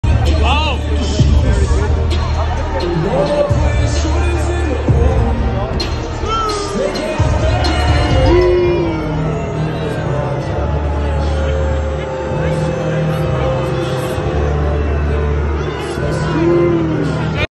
Miniminter gets the KSI Wembley Arena Show crowd to SIUUU
Simon/Miniminter/MiniminterShorts